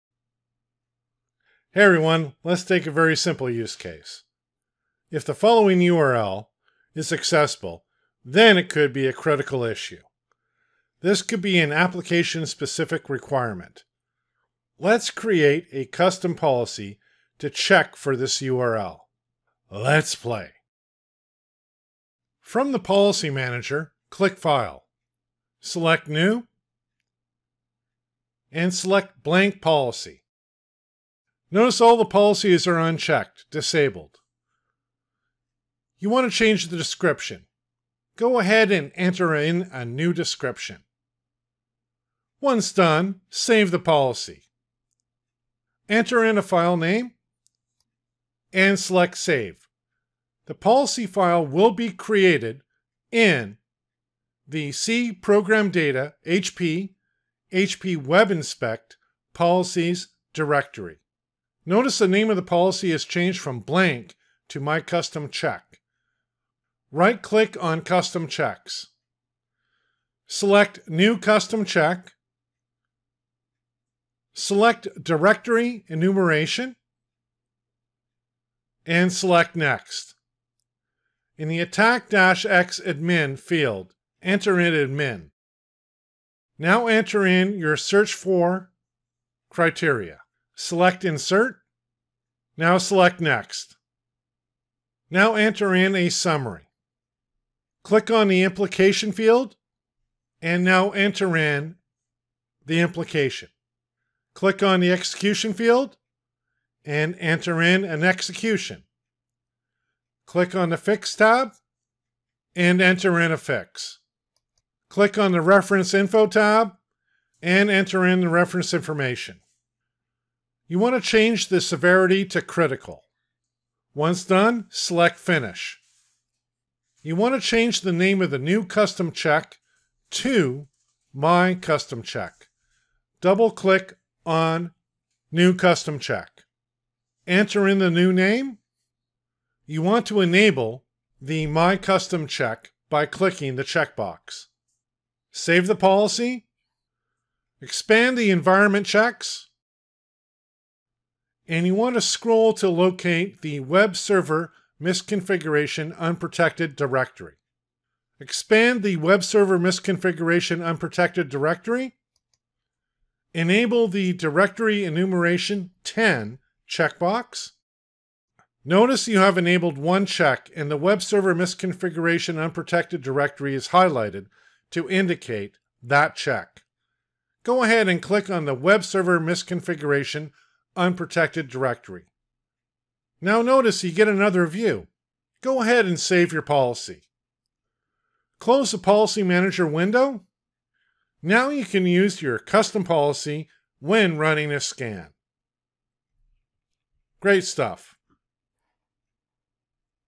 Create a Custom Policy in WebInspect
This video will show you how to create a custom policy to check for a URL.